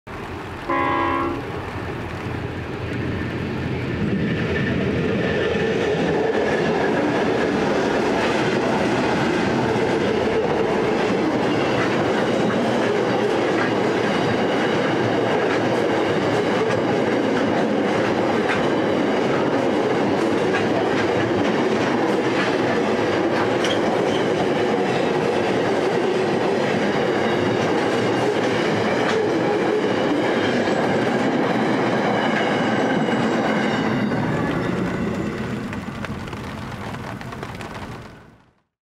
7922V SCT Dooen Container Train With CLF2 CLF4 EL62 EL60 C503 (19/7/2021) - PoathTV Railways
The Wimmera Container Lines service operated by SCT / SBR, running from Dooen (Horsham) to Melbourne, runs through the Buchter Road railway crossing and proceeds towards the dual gauge track in Batesford on a rainy winter day. C503 has been included in todays consist as it is undergoing its operational test run after being re-activated by Rail First Asset Management. Filmed from the confines of my car, the strong wind was still unfortunately able to blow rain onto the camera lens.

EL60 & EL62 have been leased from Rail First Asset Management (formerly CFCLA) to assist with a temporary motive power shortage.
Streamlined EMD locomotives CLF2 & CLF4 have recently been re-activated by their new owner, RailPower, after many years of storage.

Locomotives:  CLF2 + CLF4 +  EL62 + EL60 + C503